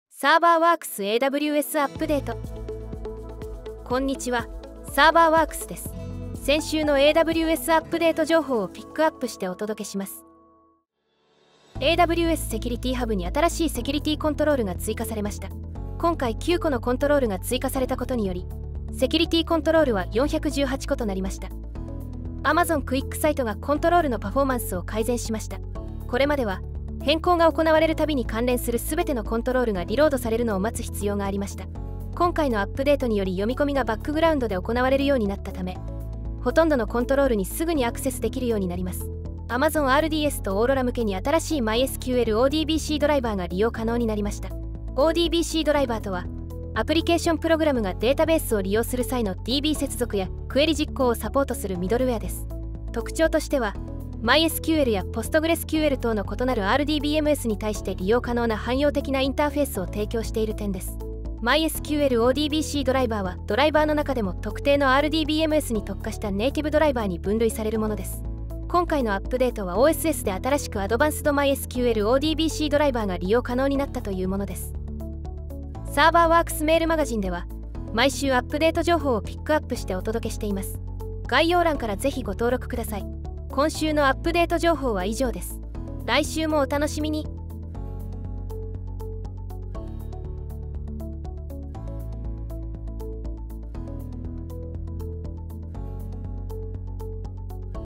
【AWSアップデート #105】AWS Security Hub に新しいセキュリティーコントロールが追加 ほか
▼ 毎週アップデート情報をお届けするメールマガジンの登録はこちらから！

■ AWS Security Hub launches 24 new security controls

■ Amazon QuickSight improves controls performance

■ New open-source Advanced MYSQL ODBC Driver now available for Amazon Aurora and RDS